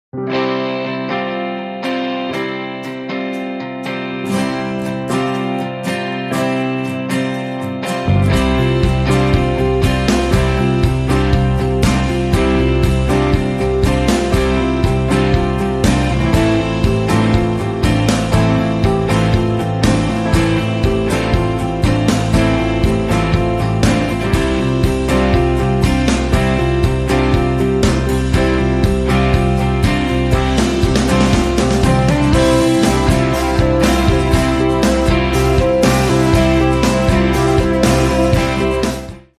Mega bridge project on Beijing–Kunming Expressway completes closure
The Weihe River Bridge, a mega extension project of the Beijing–Kunming Expressway in northwest China's Shaanxi Province has completed closure recently.